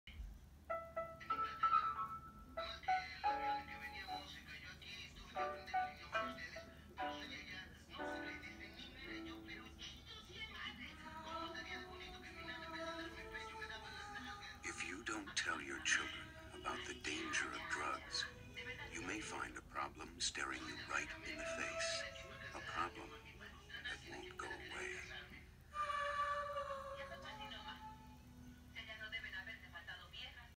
Partnership For A Drug Free America - Faces 2 (1987, USA)
It is the 21st of October and it is 10 more days till Halloween! I decided to upload this alternate version of Faces from PDFA. It doesn’t have any sheets covered and you get to see her face longer

It scared a lot of children in the 80’s except for my parents and my stepfather. Their reasons were that all of them were still in Mexico and my stepfather was about 17 at that time.